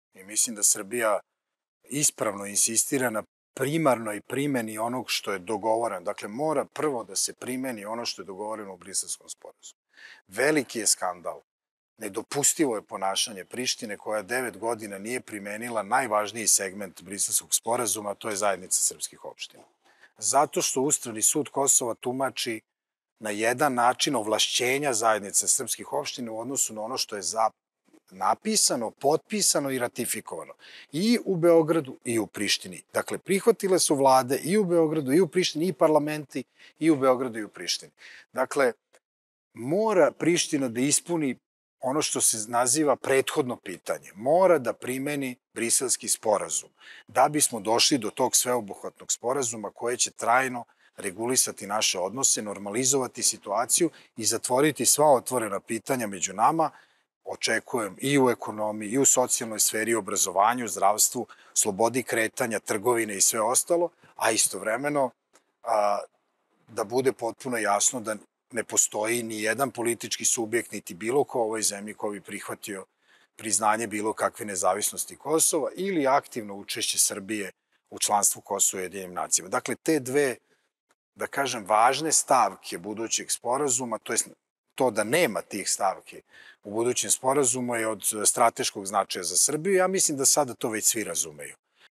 Borko Stefanović - Obaveza je Prištine da formira Zajednicu srpskih opština
Predsednik Odbora Skupštine Srbije za spoljne poslove Borko Stefanović je  ukazao da Beograd ima ogromnu odgovornost da pomogne srpskom narodu na Kosovu, ali da čuva mir i stabilnost, izbegavajući svaki sukob.